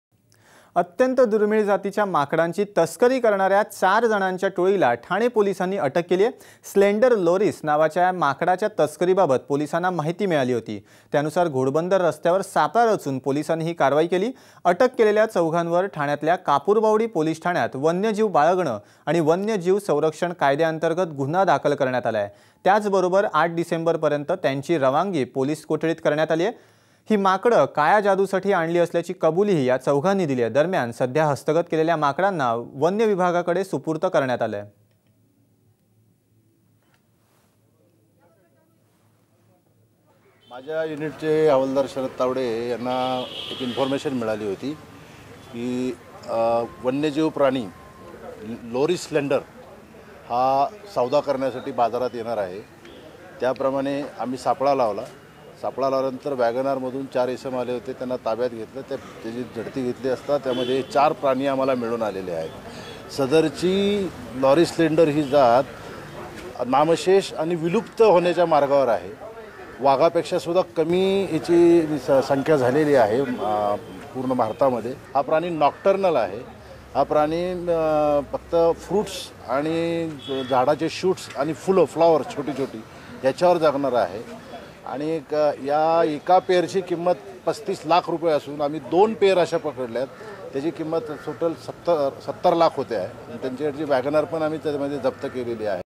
Four caught trying to sell Red Slender Loris
Four two-year-old monkeys of Red Slender Loris variety, an endangered species, were rescued by the Thane central crime detection unit.  Four people were trying to sell them near the Manpada petrol pump. The four have been arrested and produced in court.